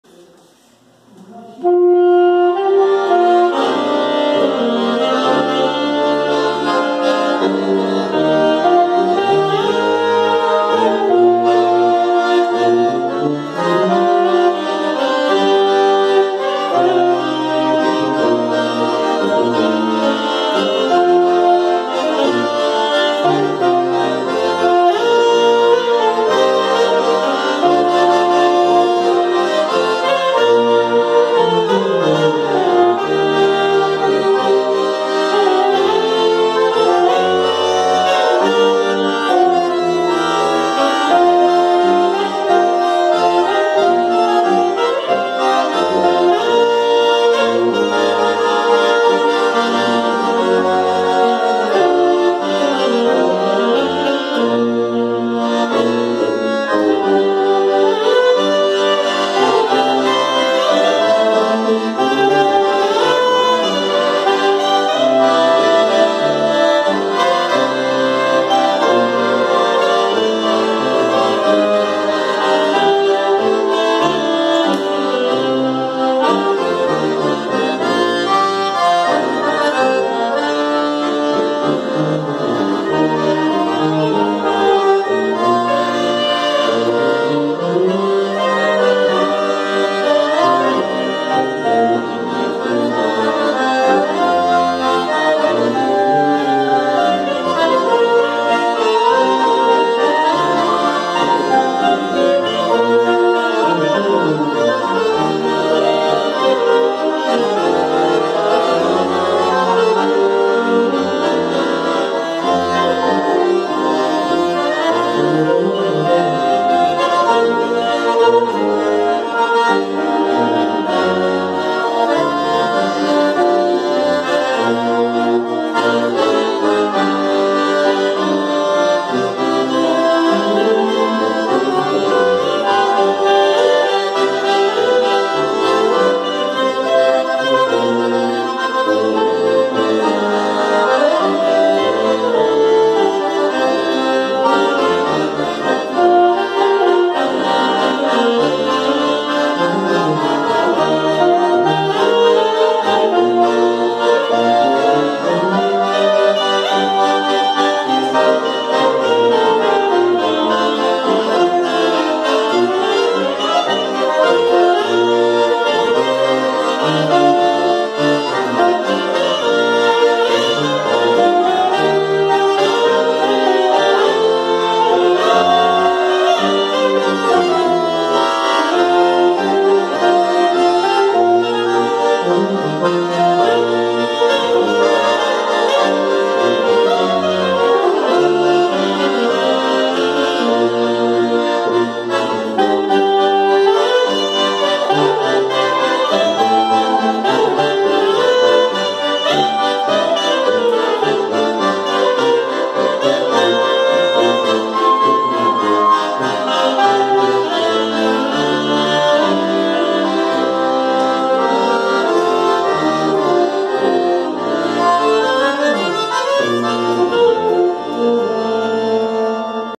Tocata em Tatuí - ( casa do nosso irmão Alexandre e família... ) - 01
Sax alto: Everton Silva,

Acordeon: Lucas Caires,

Sousafone: Samuel Rodrigues.